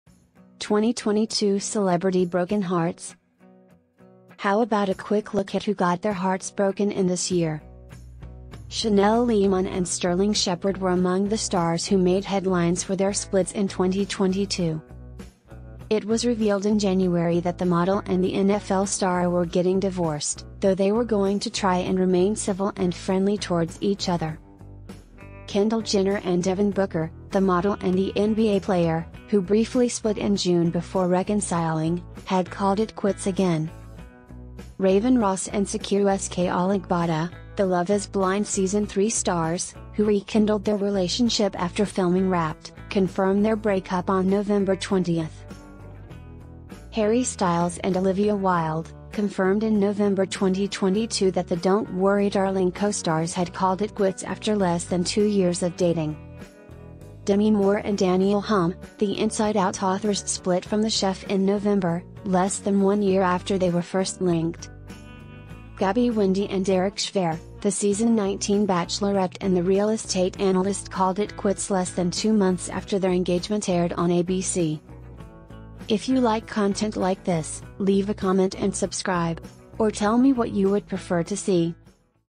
2022 Celebtity Broken Hearts.
Just a few of the breakups in 2022, if you want more let me know.

source; usmagazine